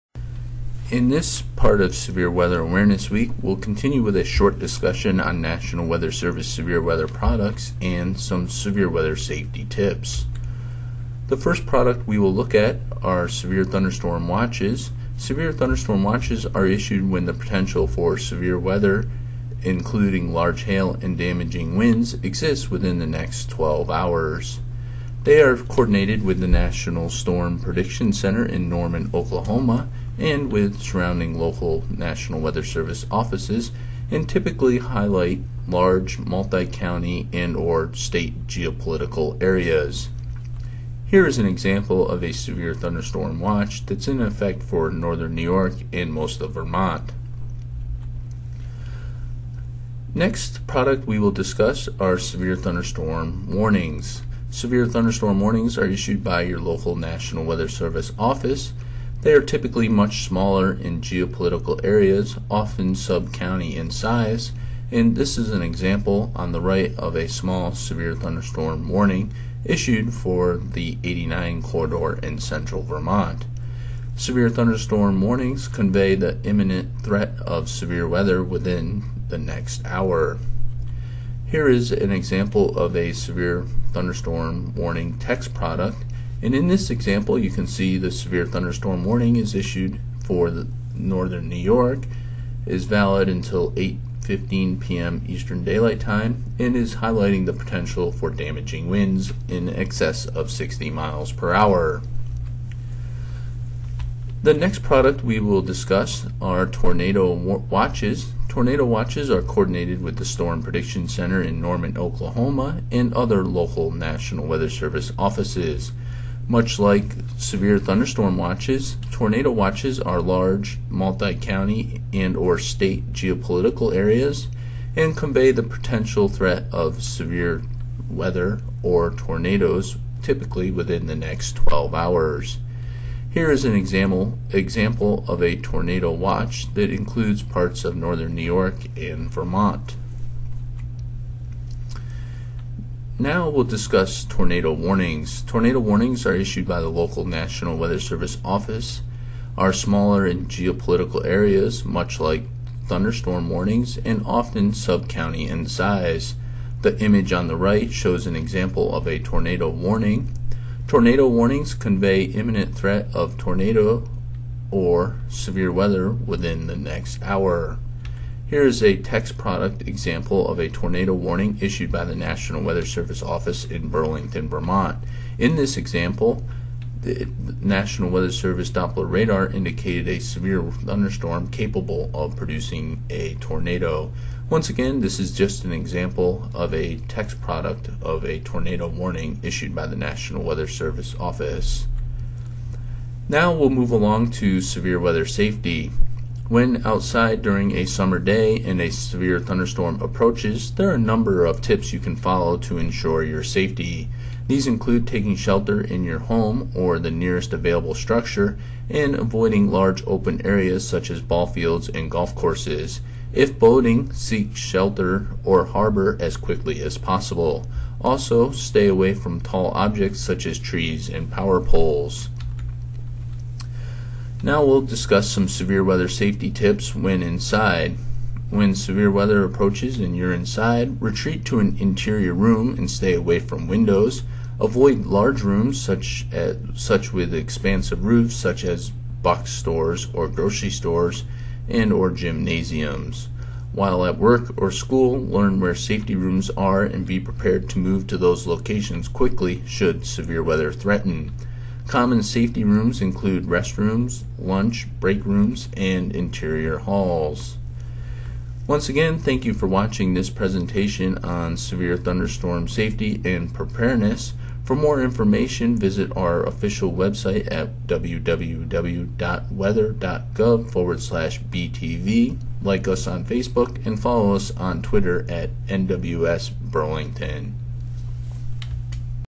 NWS Burlington Severe Weather Awareness: Thunderstorm Preparedness & Safety
Thunderstorms can be a common threat across the North Country during Spring and Summer, producing hail, heavy rain, lightning, strong or damaging winds. Watch this video on how to best prepare for them and how to stay safe.